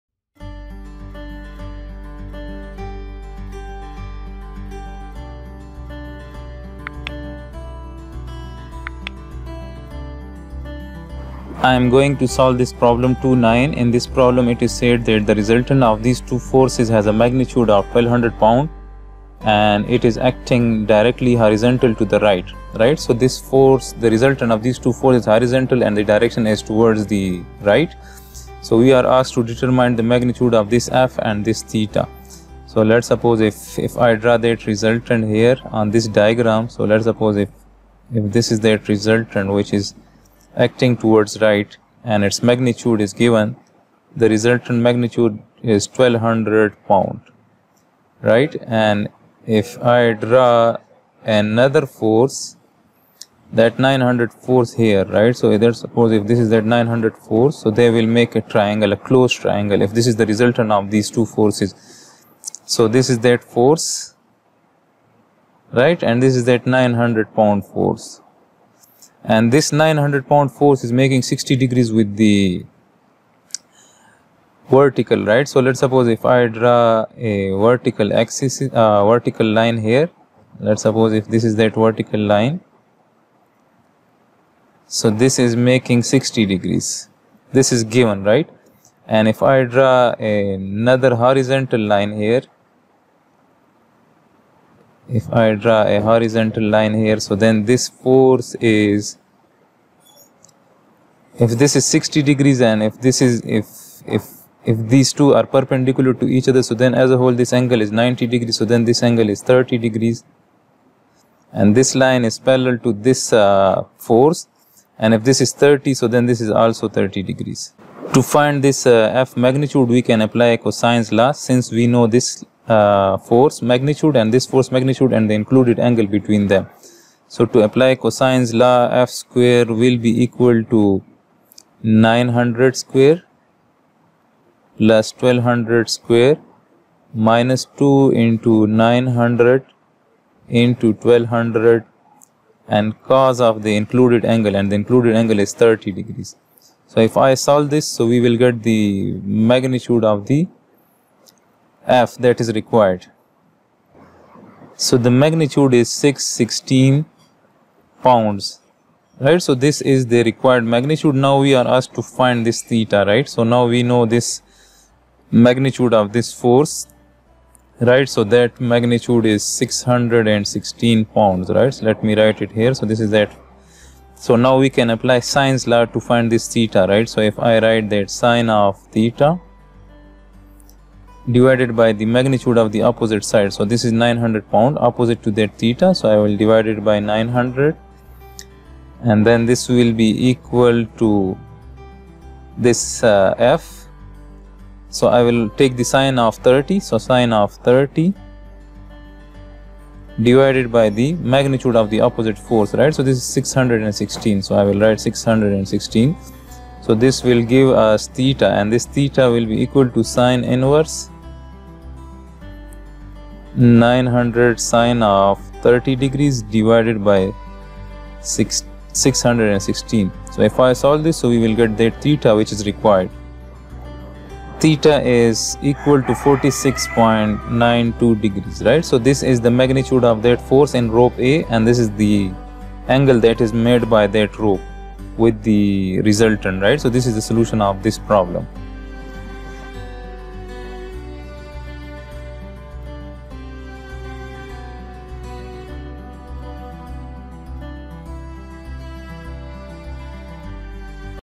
2-9 Statics Hibbeler 14th Edition (Chapter 2) | Engineers Academy
Welcome to Engineer's Academy
2-9 Statics Hibbeler 14th Edition (Chapter 2) | Engineers Academy
Chapter 2: Force Vectors
Problem Solution:
2–9. If the resultant force acting on the support is to be 1200 lb, directed horizontally to the right, determine the force F in rope A and the corresponding angle u.